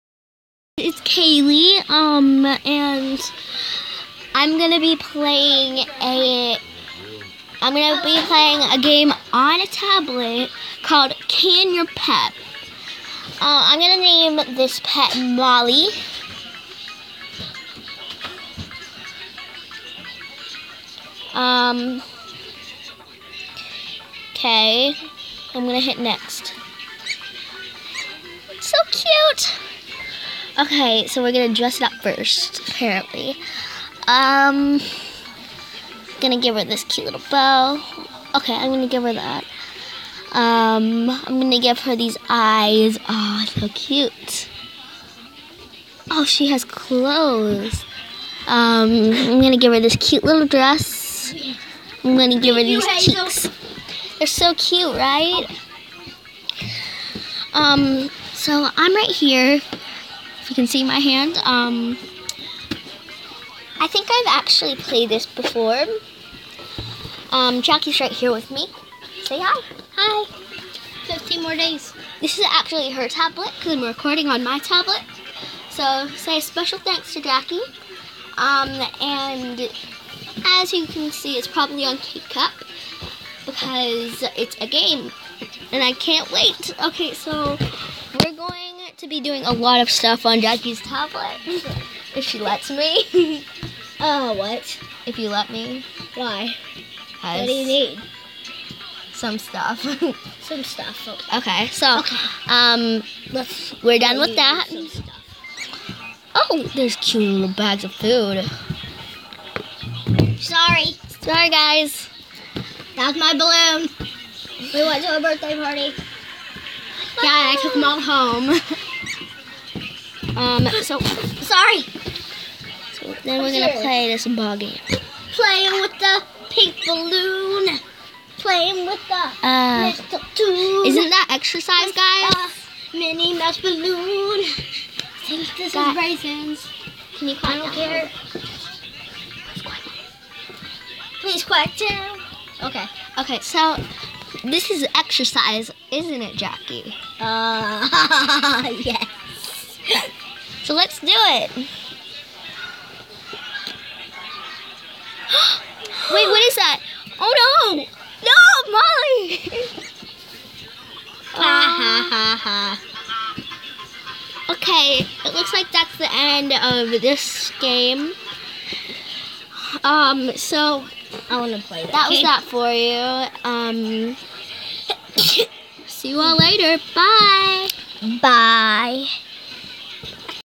I caned my pet 😳😟
December 12 2015 all i have to say is this game is weird i played it on a samsung tablet